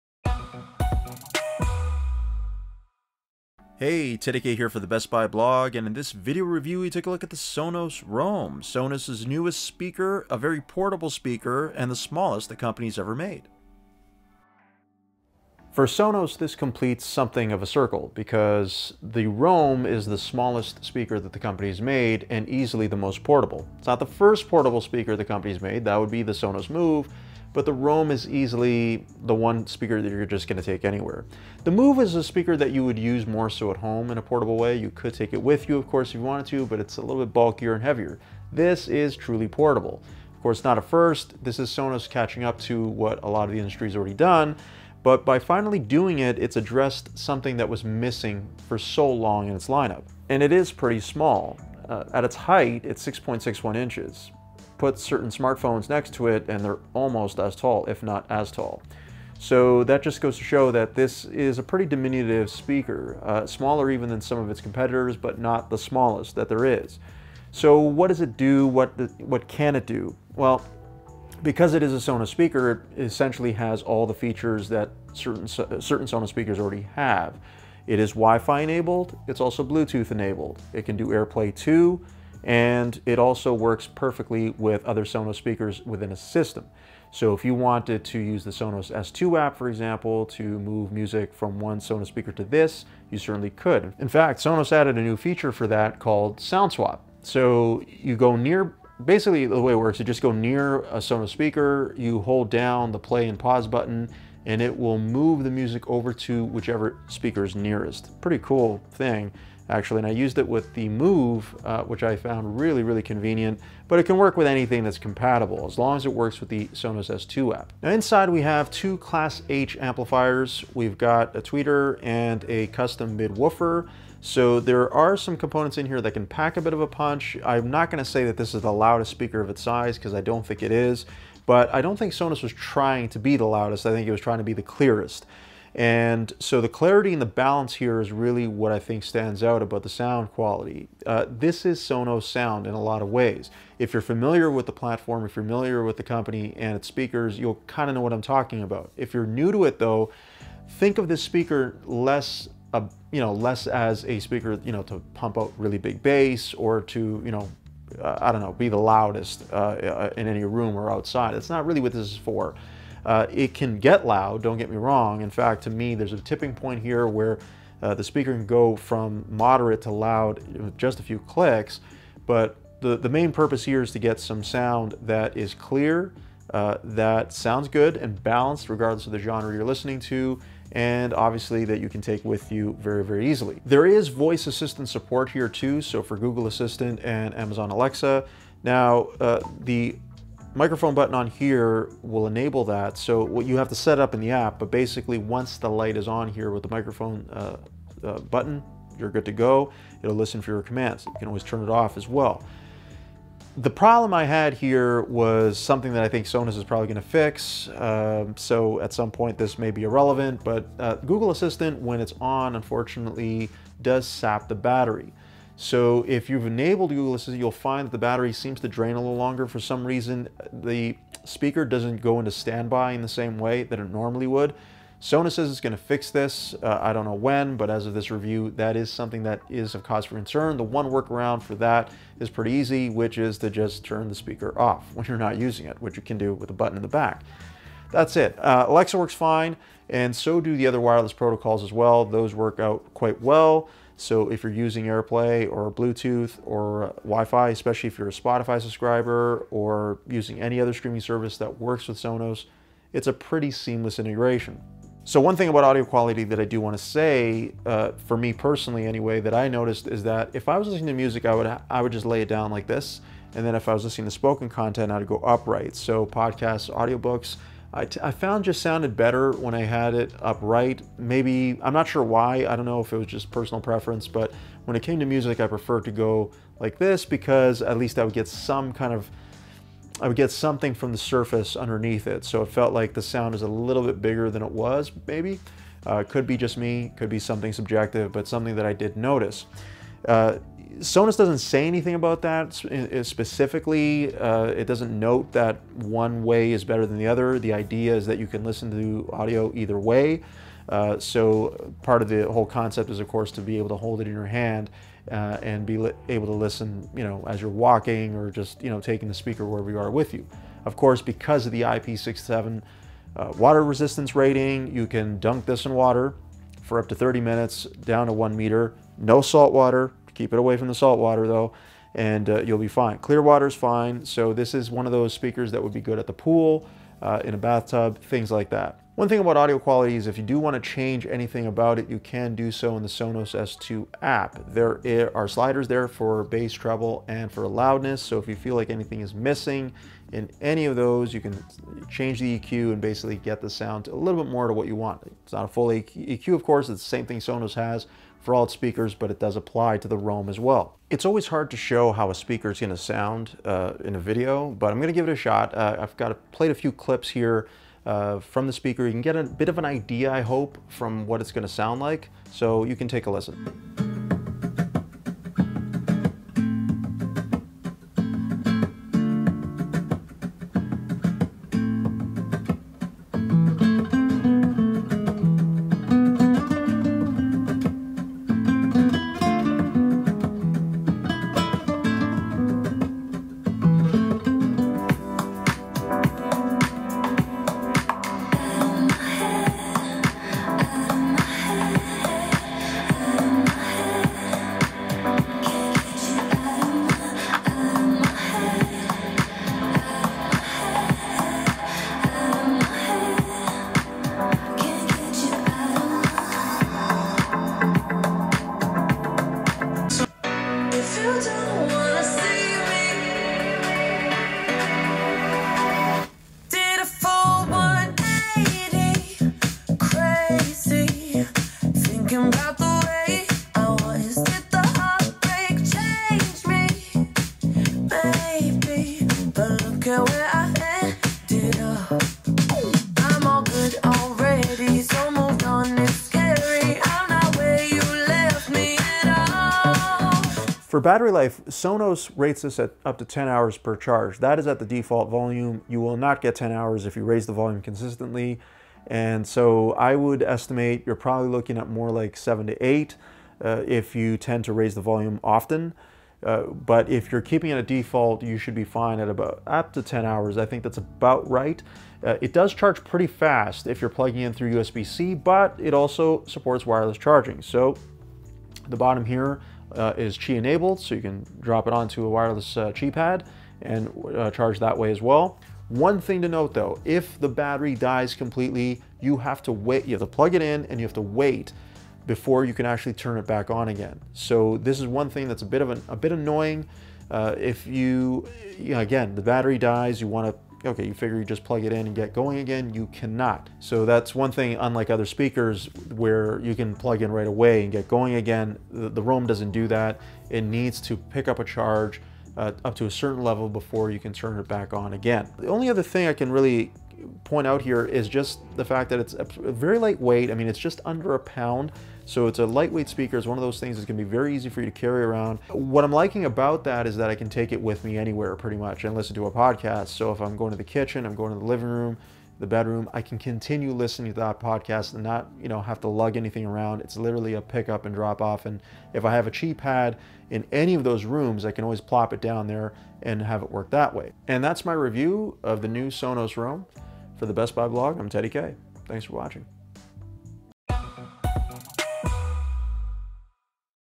Sonos Roam Hands-on Review
The Sonos Roam finally puts the company into new territory of portable speakers that you can take just about anywhere to play big sound when you need it to. My review.